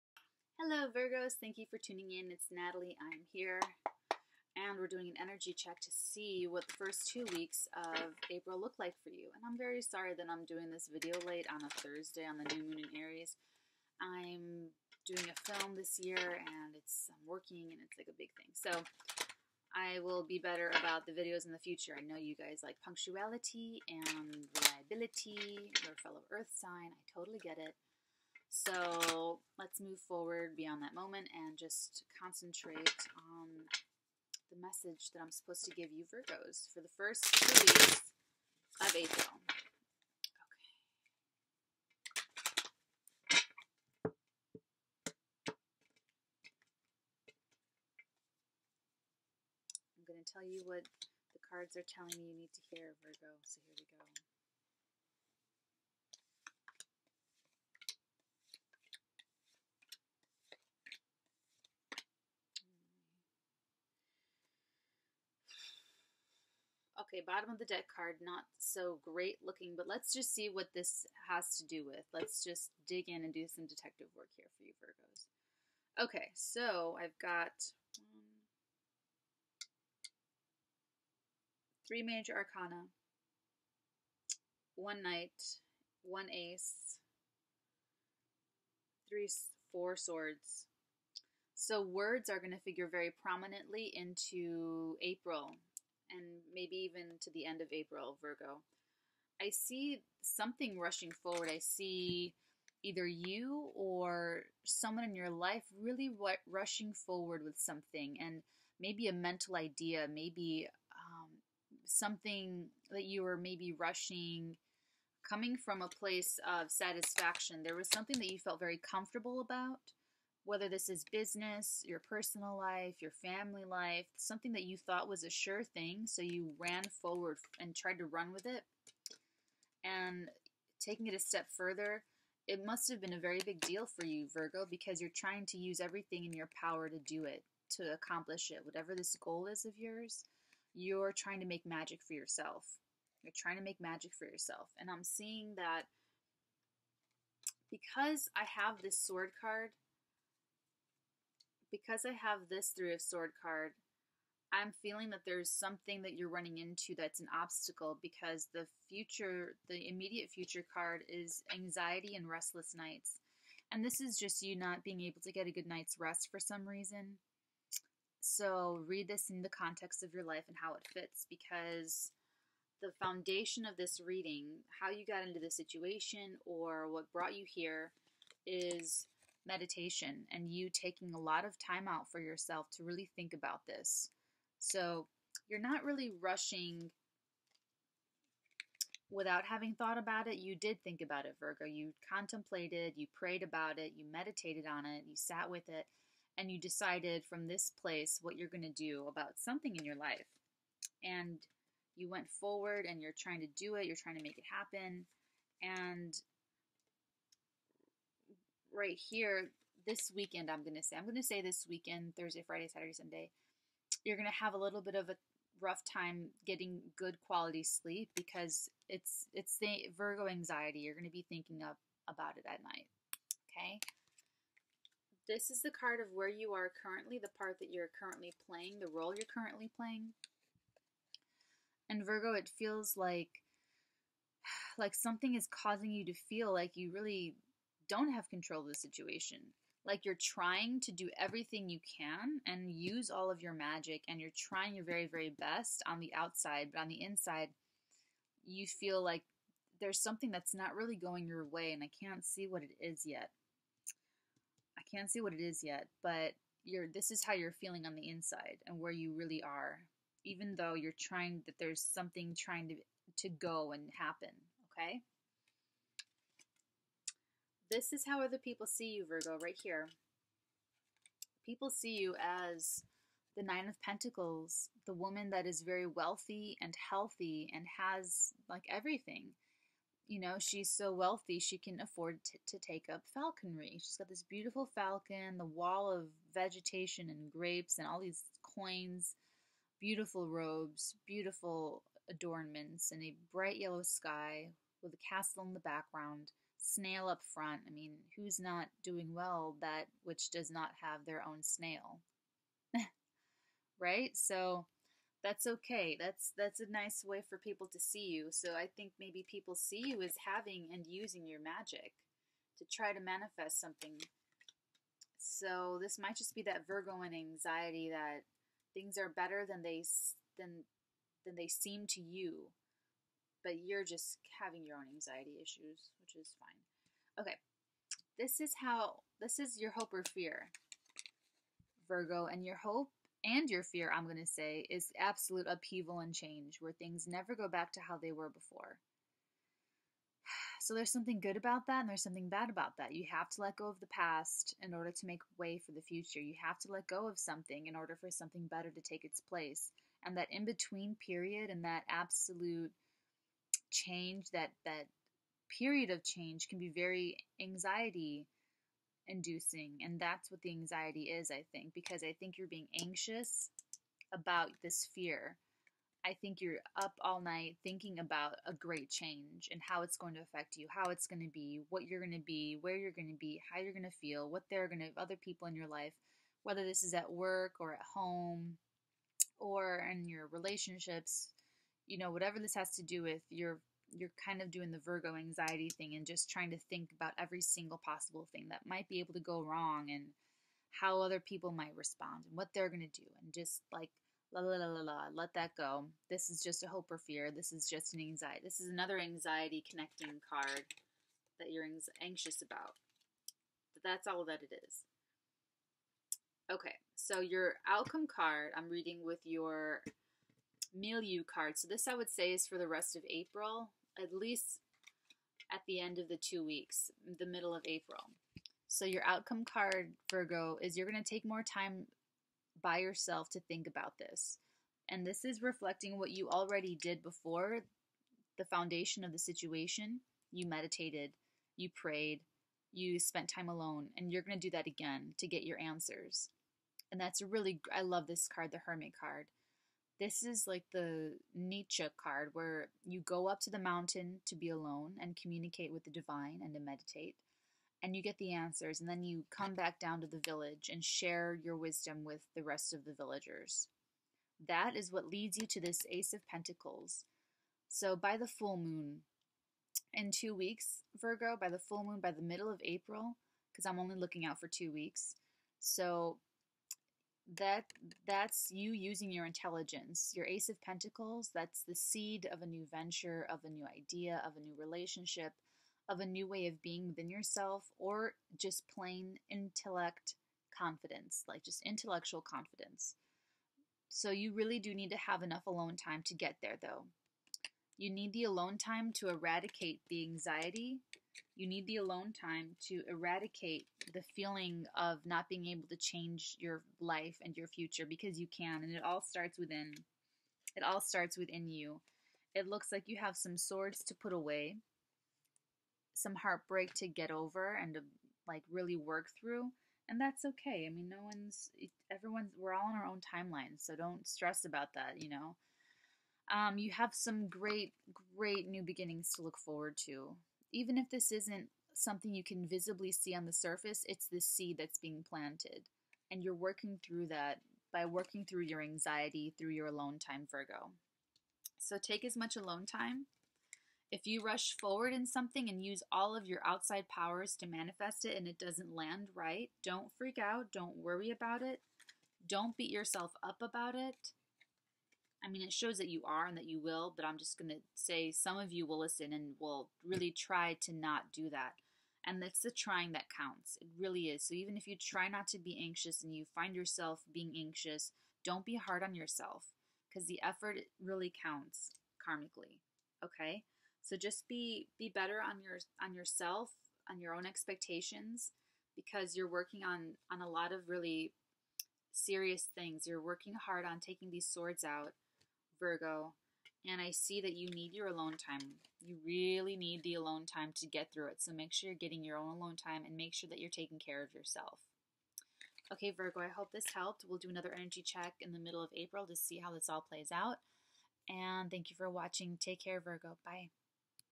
VIRGO APRIL 1/15 2019! BRIGHT BEGINNING HAS BEEN INITIATED!
Celtic Cross Tarot Spread Energy Check for Virgo and Virgo Risings April 1 - 15th, 2019.

Check for your rising sign.

The effort counts karmically, Virgo. So don't be so hard on yourself. You're working hard on healing and taking the swords out. This takes time and nurturing so please take more of the alone time in order to do the work the right way, Virgo. You may want to rush forward, but the cards are telling me that you have been making the most progress in your alone time when you're being positive with yourself. Keep this practice whether it's prayer, meditation, etc. because the seeds are there and they need nurturing to grow strong. So sorry this is late, Virgo! Forgive me!